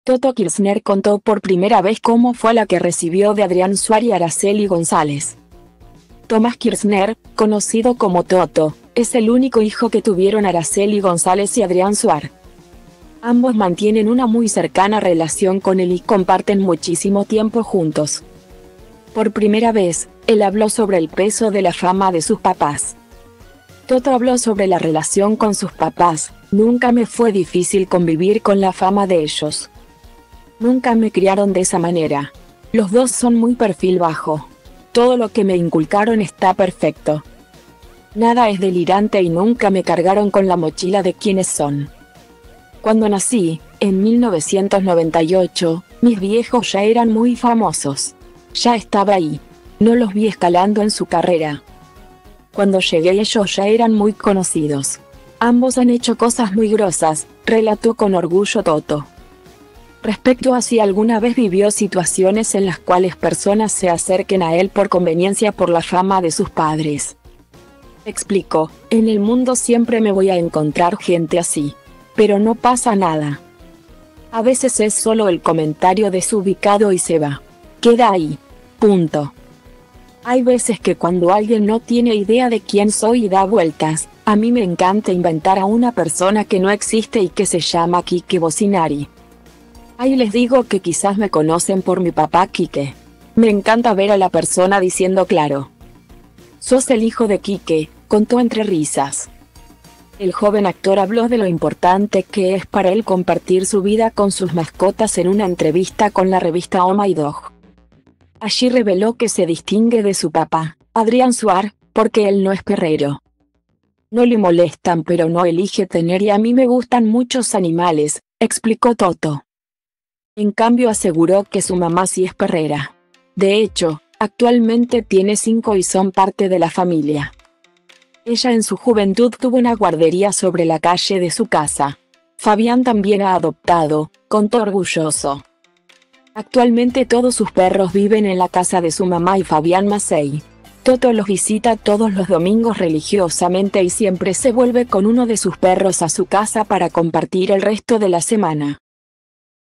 Toto Kirzner contó por primera vez cómo fue la que recibió de Adrián Suar y Araceli González
Tomás Kirzner, conocido como Toto, es el único hijo que tuvieron Araceli González y Adrián Suar.Ambos mantienen una muy cercana relación con él y comparten muchísimo tiempo juntos.Por primera vez, él habló sobre el peso de la fama de sus papás.